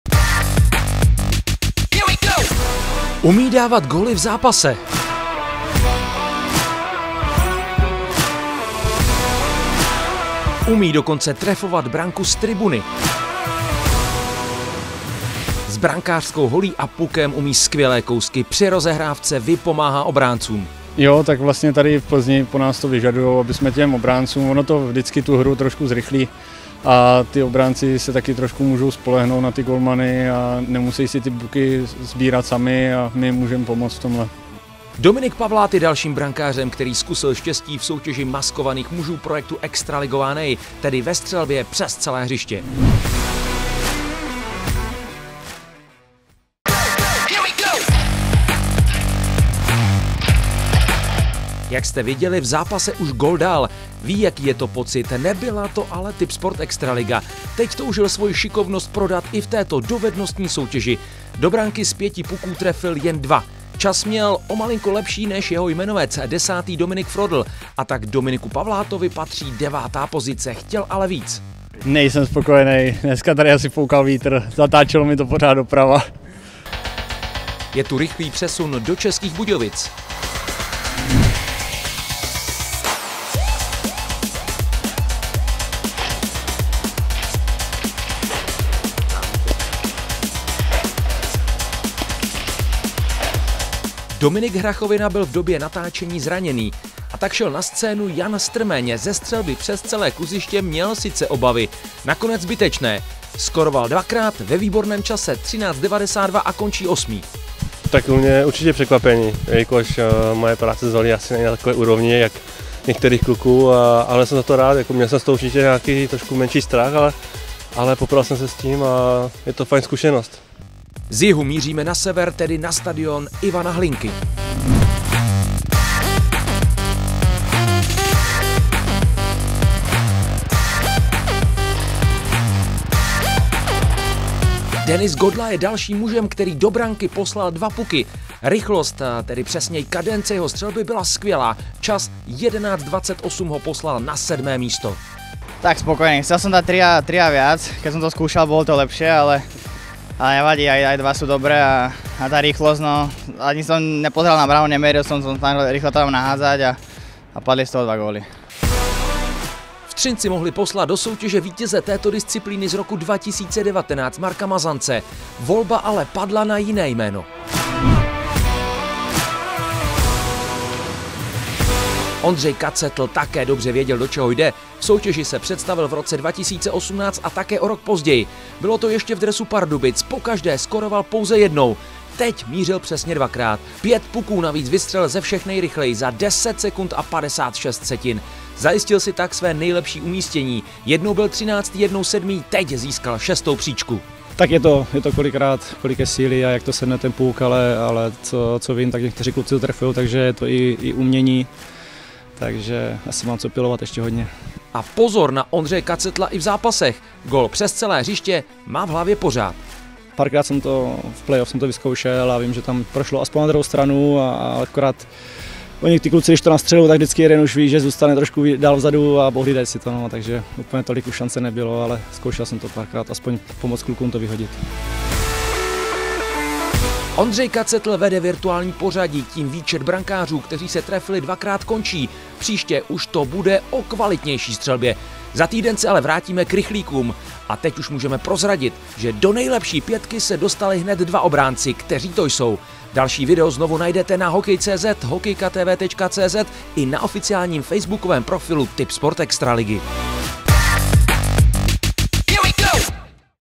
V zápase už skóroval. Jak mu to šlo na prázdném ledě?
Kdo z brankářů má nejlepší střelu, kdo umí zamířit? Maskované muže testujeme v projektu Extraligová NEJ soutěží, v níž střílí přes celé kluziště. V dalším díle jde do akce i gólman, který už si pocit střelce prožil přímo v zápase. Jak mu to šlo na prázdném kluziště, proč nebyl spokojený se svým výkonem, kolik puků poslal do prázdné branky?